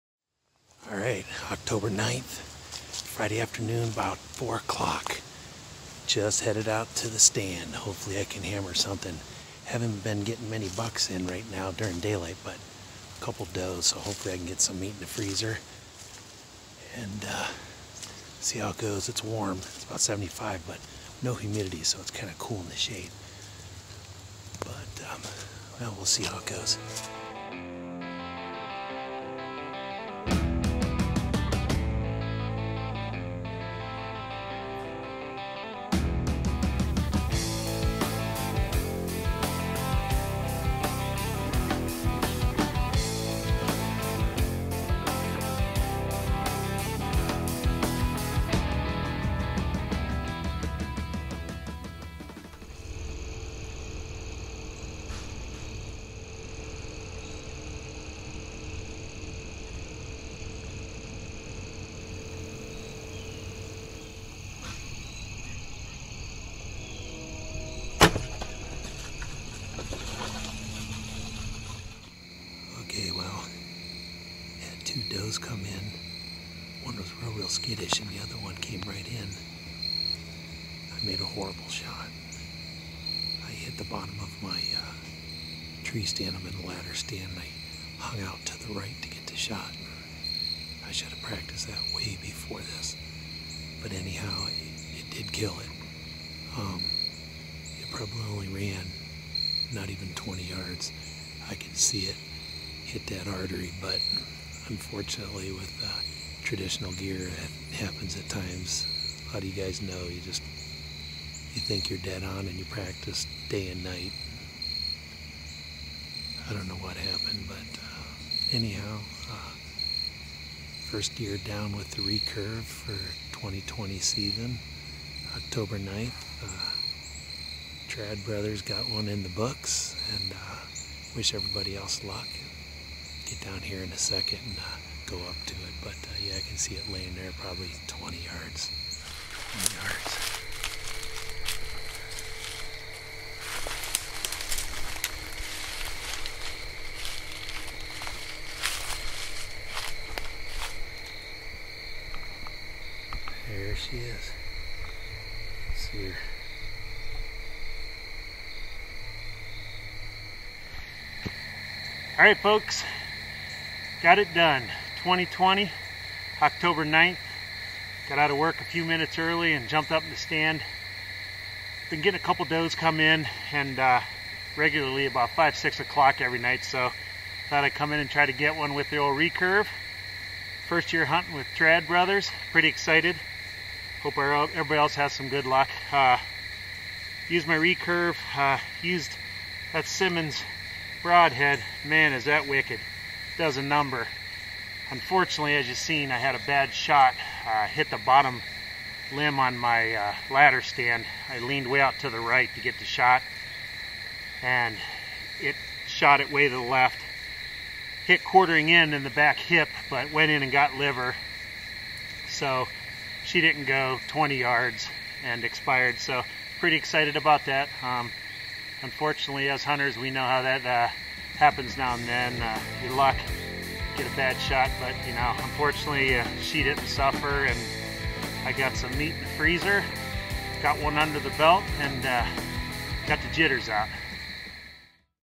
Trad Brothers- Traditional Bowhunt Ohio doe
Team member Larry connects on a early season doe in his home state of Ohio. Even on a marginal shot the Simmons broadheads put her down quick!!